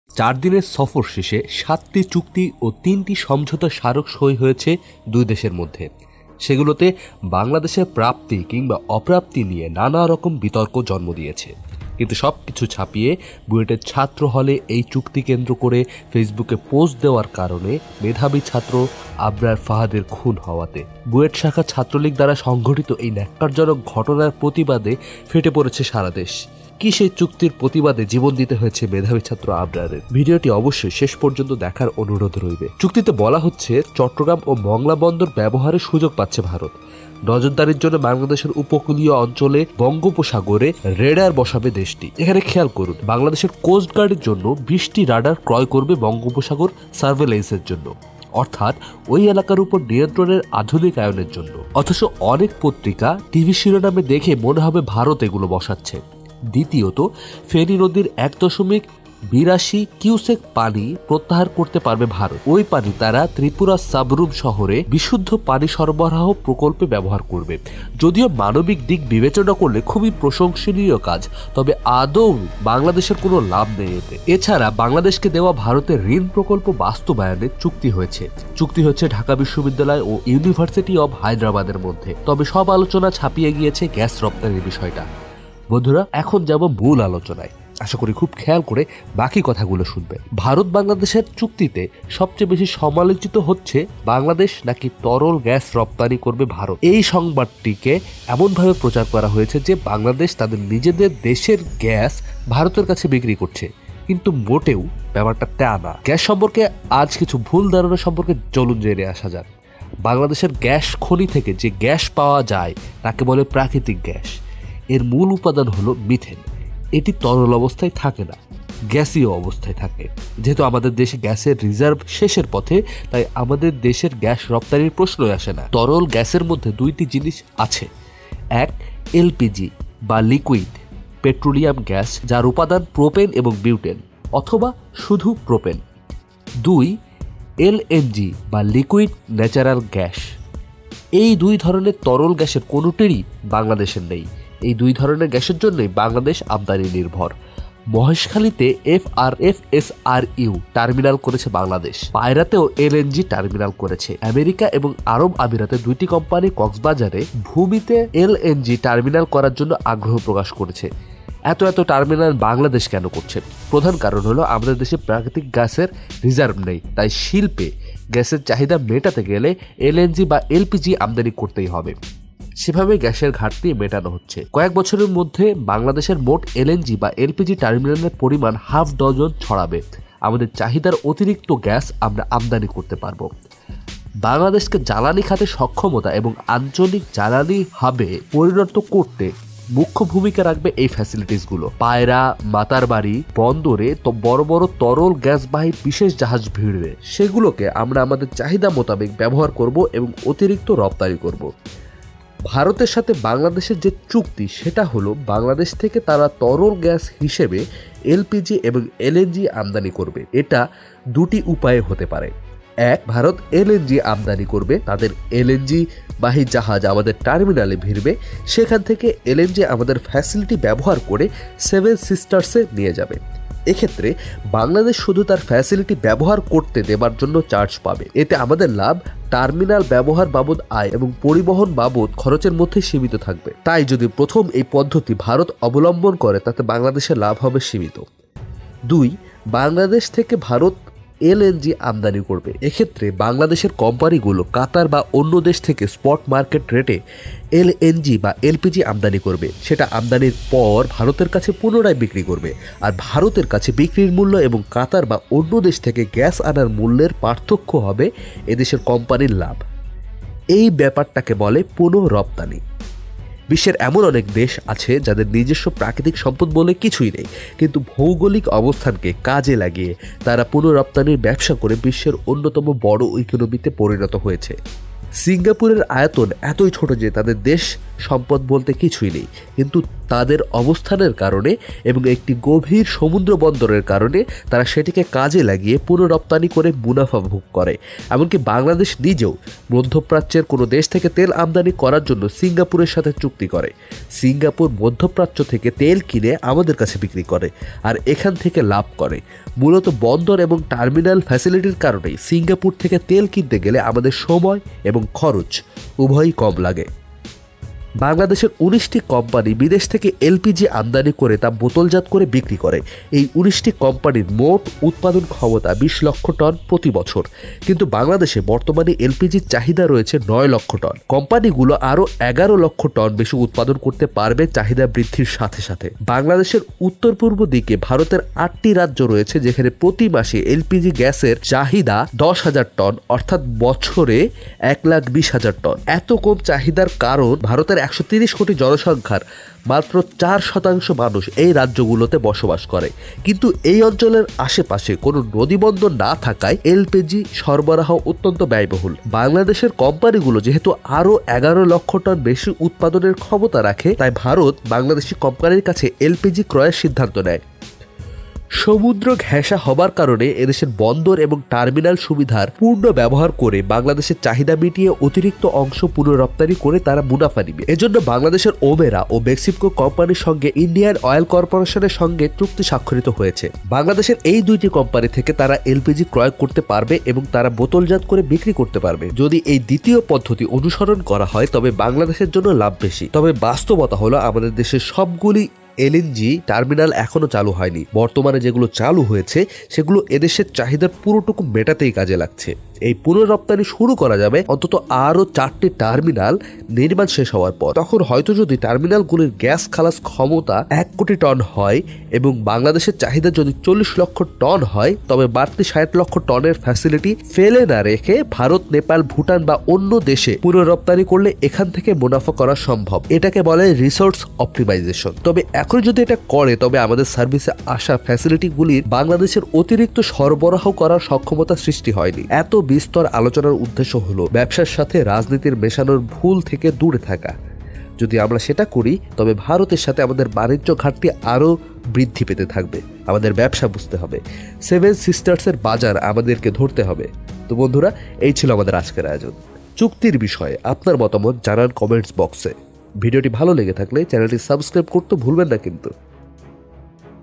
ভারত-বাংলাদেশ চুক্তি || যা আছে চুক্তিতে (মাথা ঘুরে যাবে) || বাংলাদেশের কতটা লাভ হবে?! Trendz Now
দিল্লিতে ৭ চুক্তি ও সমঝোতা স্মারক সই করেছে বাংলাদেশ ও ভারত। প্রধানমন্ত্রী শেখ হাসিনা ও ভারতের প্রধানমন্ত্রী নরেন্দ্র মোদির উপস্থিতিতে সই করে দুই দেশ। একই সঙ্গে ভিডিও কনফারেন্সের মাধ্যমে বাংলাদেশে তিন প্রকল্পের উদ্বোধন করেন দুই প্রধানমন্ত্রী। আলোচনায়-সমালোচনায় সবচেয়ে বেশি কথা উঠেছে এইবারের চুক্তি নিয়ে। চলুন জেনে আসা যাক.. কি অআছে সেই চুক্তিতে.. কেন মরতে হলো আবরারকে